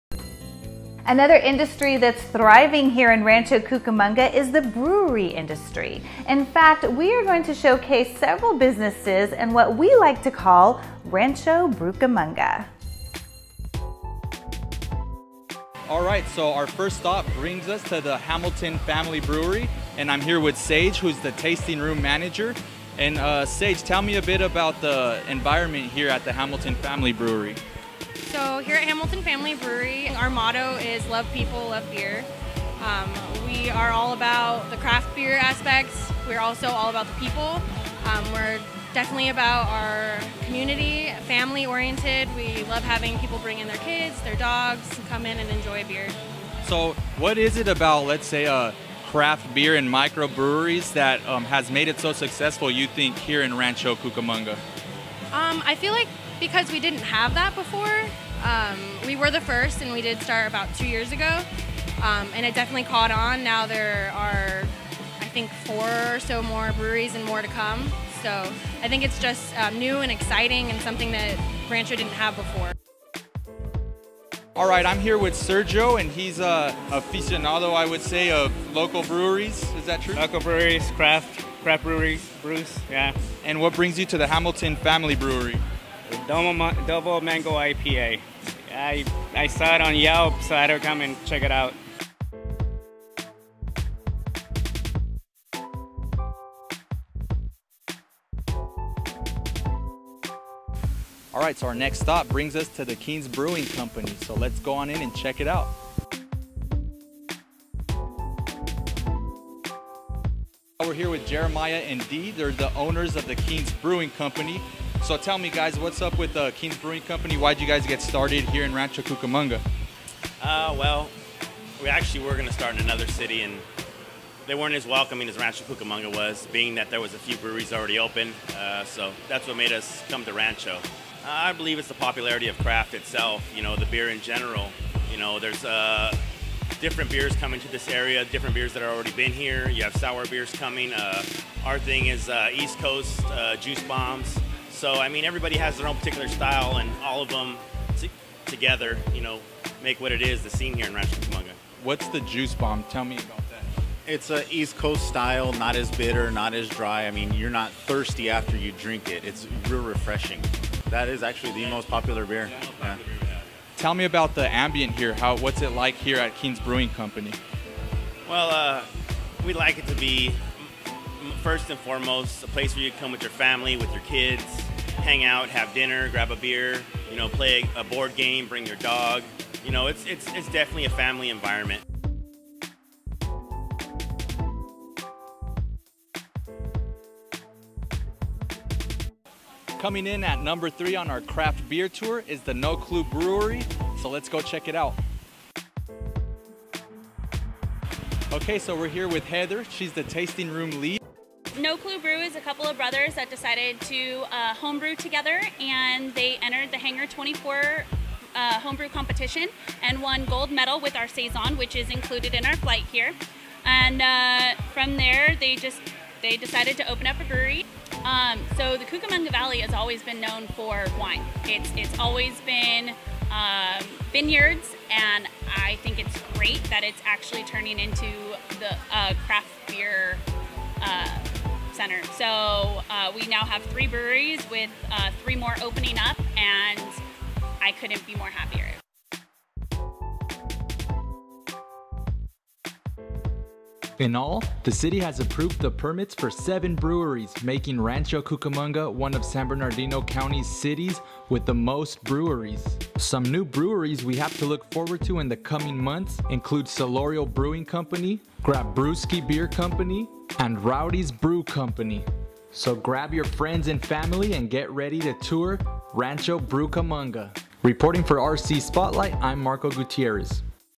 RC Spotlight: A tour of some of Rancho "BrewCamonga's" craft beer hot spots
Rancho Cucamonga's brewery industry is thriving, earning it the nickname “Rancho BrewCamonga.” In this story, RC Spotlight's Marco Gutierrez tours Hamilton Family Brewery, Kings Brewing Company and No Clue Craft Brewery.